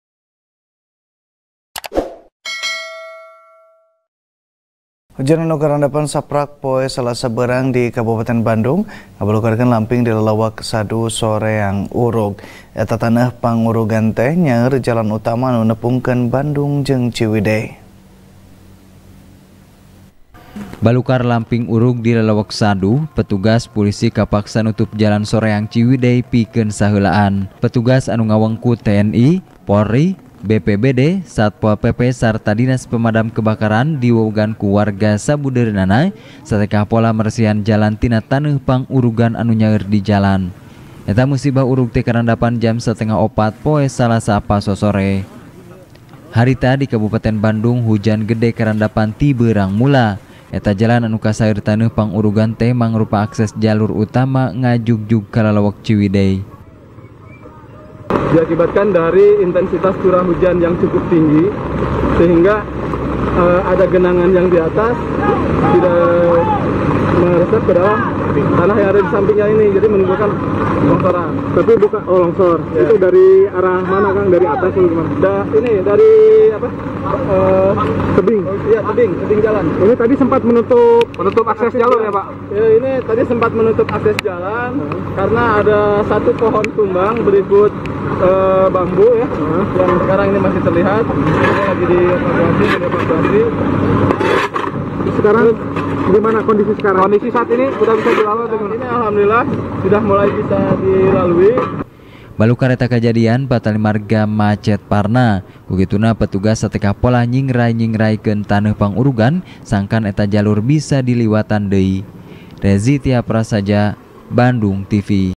Hujan Gede, Lamping Urug Nyaeur Jalan Anu Nepungkeun Bandung | TP | BADNUNGTV
TP | BANDUNGTV | DERI FIRMAN | Hujan Anu Karandapan Saprak Poe Salasa Beurang, Di Kabupaten Bandung, Ngabalukarkeun Lamping Di Lelewek Sadu, Soreang, Urug. Eta Taneuh Pangurugan Teh Nyaeur Jalur Utama Anu Nepungkeun Bandung Jeung Ciwidey.